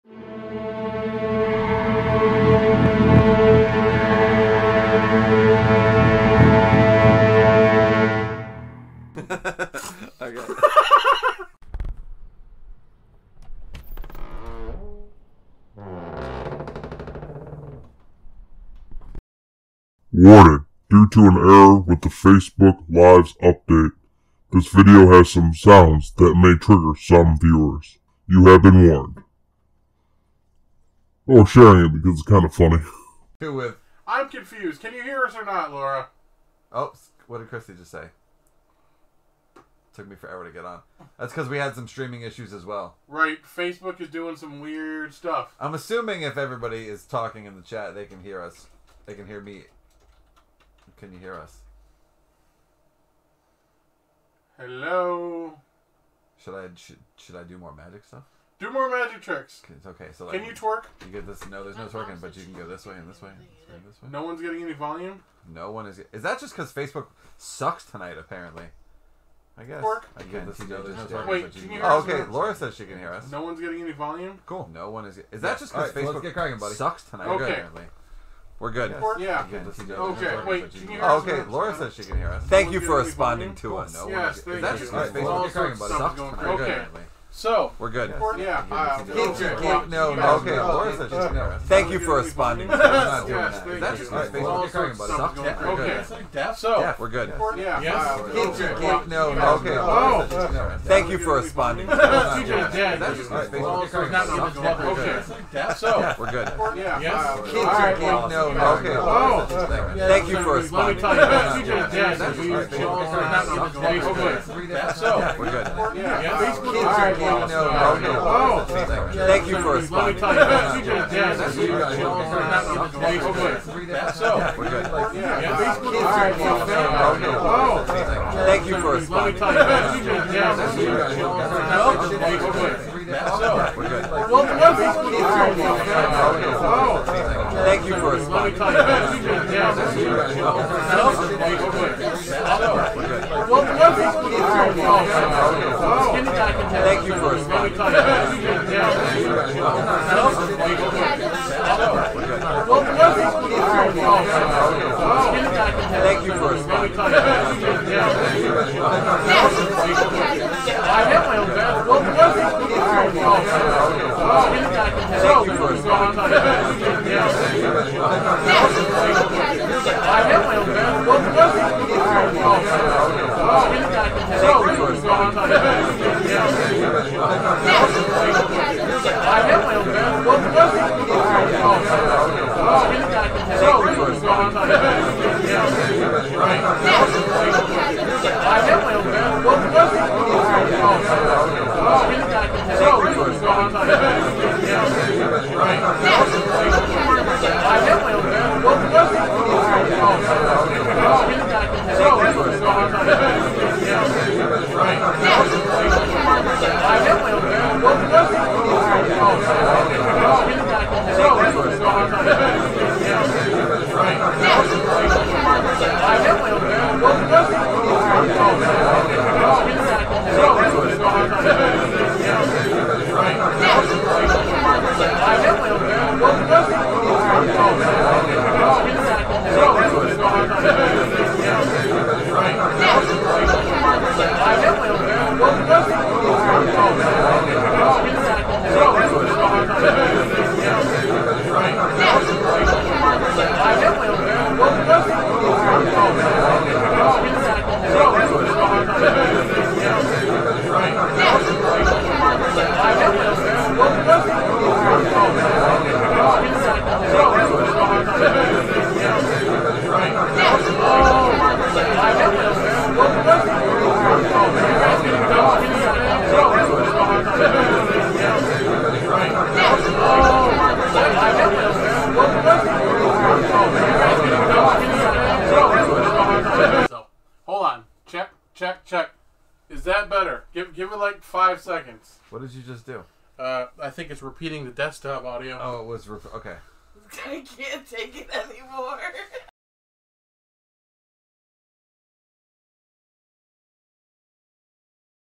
Nightmare Fuel - Dream 1
In the Fat Guy Skinny Dude Universe, Nightmare inducing offenses are considered especially heinous. In Northern Michigan, the dedicated detectives who investigate these vicious fiascos are members of an elite squad known as the Nightmare Fuel Unit. These are their stories.

Or listen to our 1st two seasons on Spotify 🎙